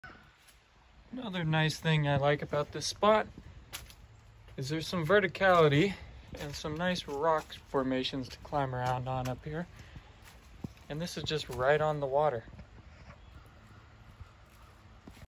Video of Natural Dam, AR from Cultivating Fire L.
▶︎ Small waterfall with a gravel lot
We found this quiet spot with a small waterfall on Mountain Fork Creek that has a fairly large (semi-level) gravel parking lot right next to the natural waterfall which gives good noise cover for the road nearby. There are a few small trails along the creek and a few small bluffs with climbing spots for those who like some verticality.
Not great for solar panels but we had a good view of the north sky so Starlink works great here!
Also, a few stray dogs and cats came to see us a couple times.